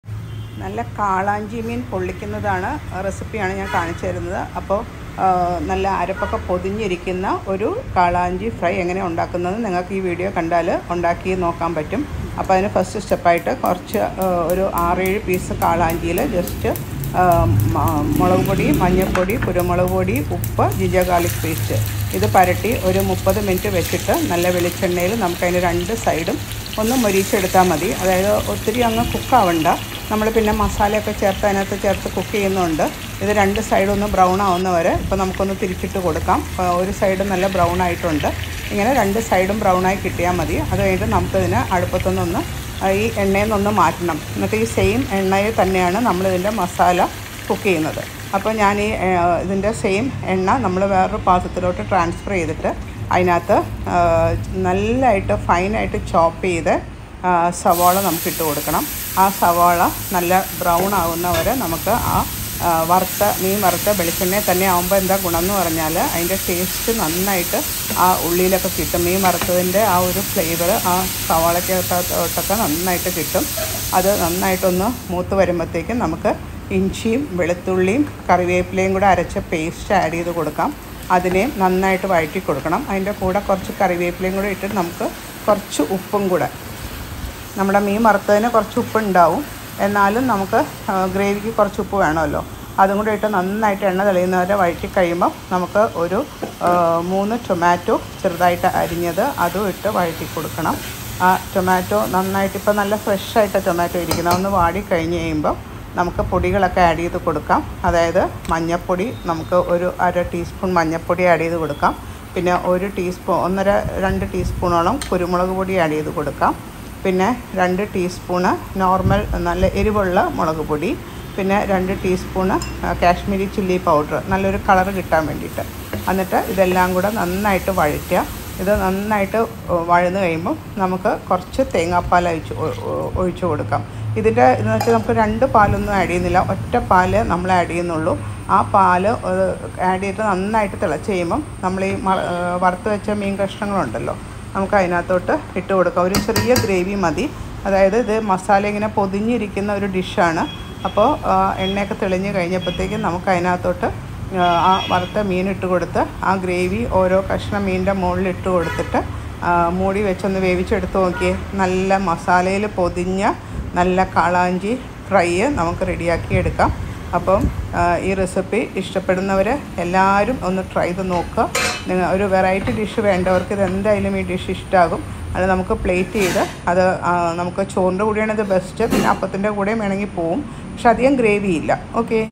Fish Pollichathu Malayalam || Fish Pollichathu Recipe
This is one of my special fish recipe- Fish Pollichathu. If you are a fish lover, definitely try out this recipe and please give me feedback.

Ingredients
Fish pieces. 11/2 kg
Salt
Turmeric.  1/2tsp
Coconut oil.  1cup
Chopped onion.  4
Curry leaves
Turmeric.  1/2tsp
Pepper powder.  2tsp
Kashmiri chilly powder.  2tsp
Chilly powder.  2tsp
Salt
Thick coconut milk.  2cup
Method
Marinate fish with salt, turmeric, chilly and pepper for 30 minutes. Heat coconut oil, shallow fry fish till brown on both sides. Remove fish from the oil. In the same oil, sauté onion till brown. Add curry leaves, ginger garlic paste and sauté well. Add salt , tomato. Sauté till tomato is soft.  Add all powder and sauté. Add coconut milk. Allow to simmer. Dried fish and cook till masala is well coated on fish. Serve hot.